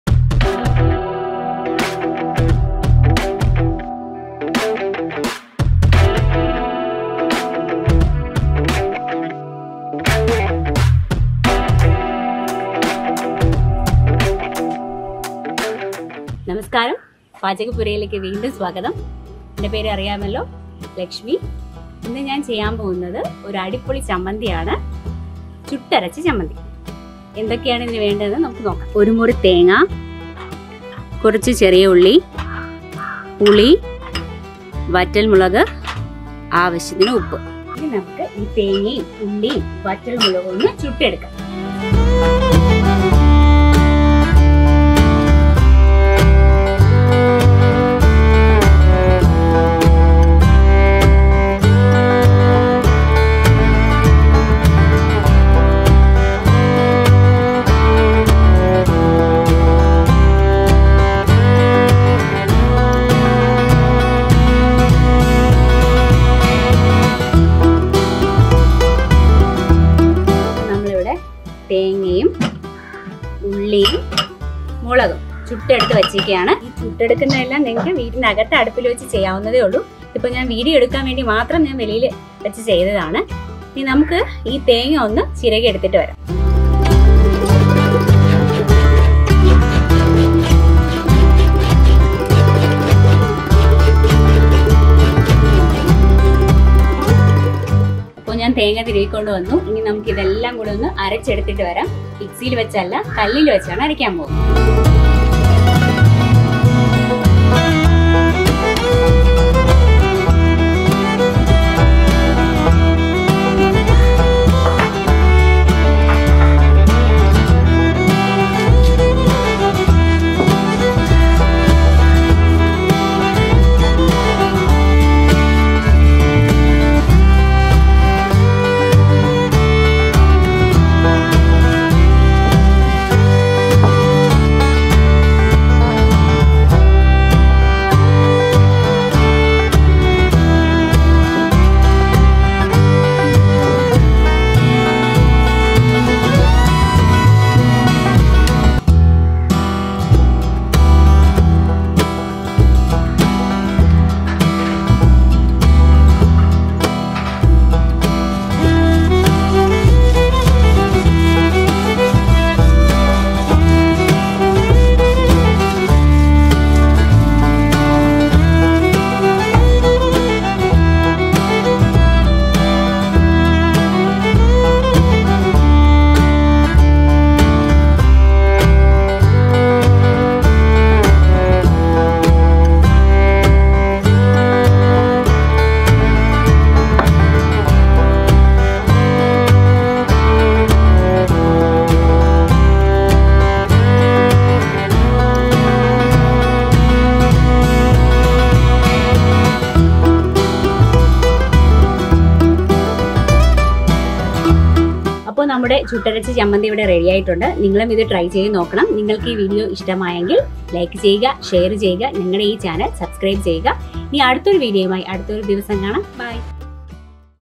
Chuttaracha chammanthi | ചുട്ടരച്ച ചമ്മന്തി | ചുട്ടരച്ച തേങ്ങാ ചമ്മന്തി
ഇന്നത്തെ recipe തീയിൽ ചുട്ടരച്ച തേങ്ങാ ചമ്മന്തി ആണ്. ചൂട് കഞ്ഞിയോടൊപ്പം കൂട്ടി കഴിക്കാൻ ഒരു നല്ല സൈഡ് ഡിഷ്‌ ആണ് ഈ തേങ്ങാ ചമ്മന്തി.
ഒരുമുറി തേങ്ങയും ഉള്ളിയും വറ്റൽ മുളകും തീയിൽ ചുട്ട്. ബാക്കി വീഡിയോയിൽ. 😁